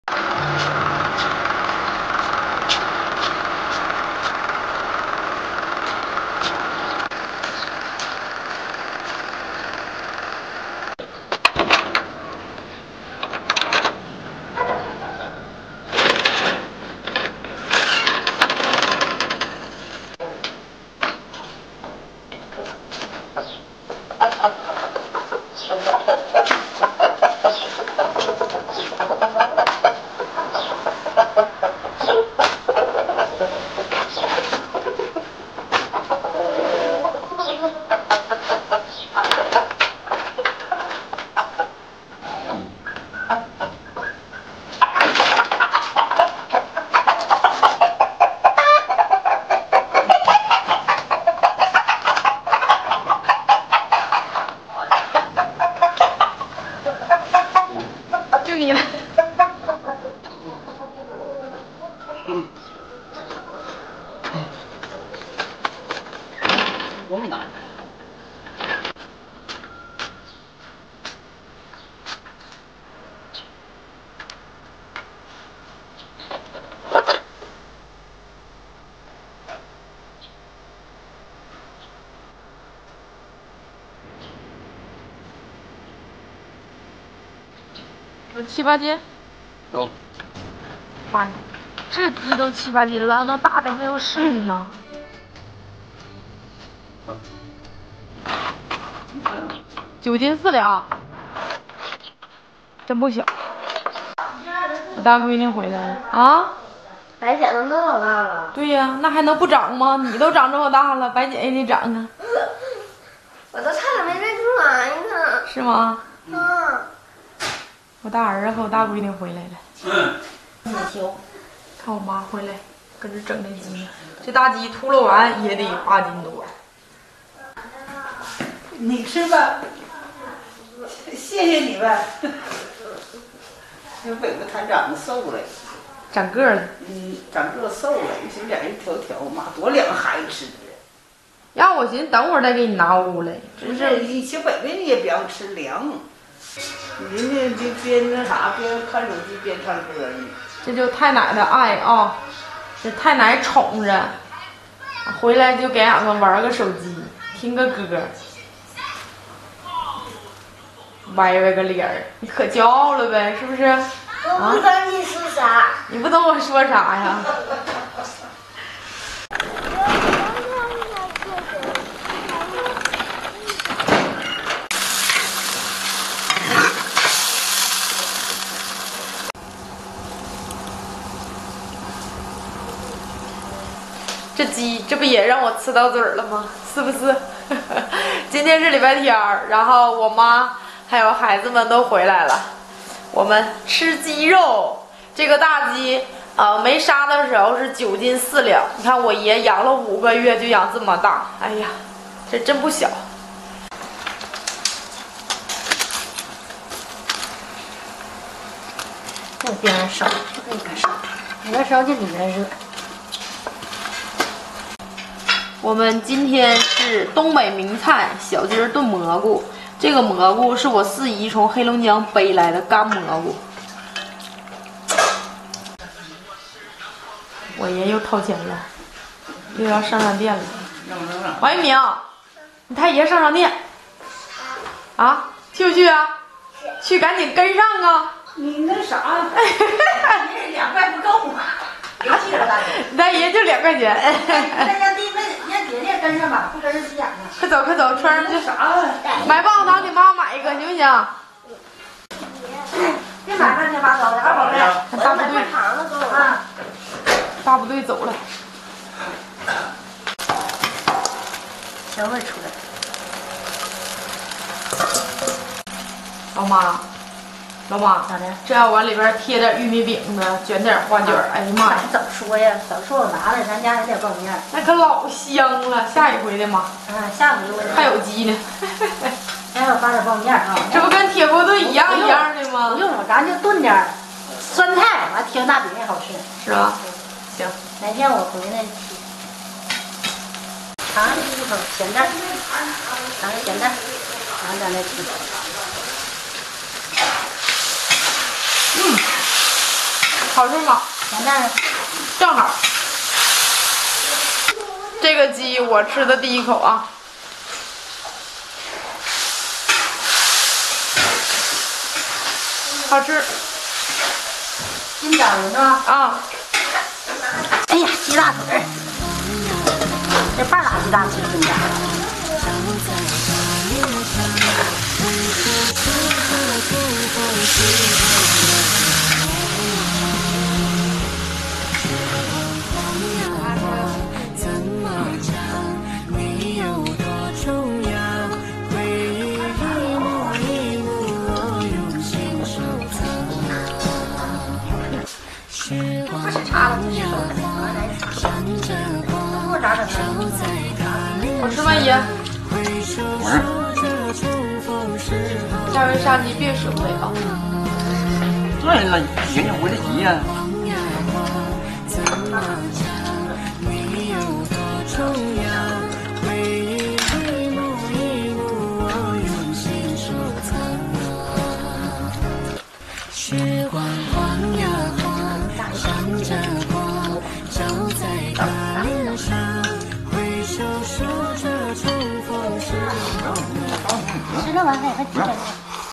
双双盼望已久的鸡，只等重孙回家终于安排上了，团聚吃肉太幸福
欢迎来到《于双双》的小世界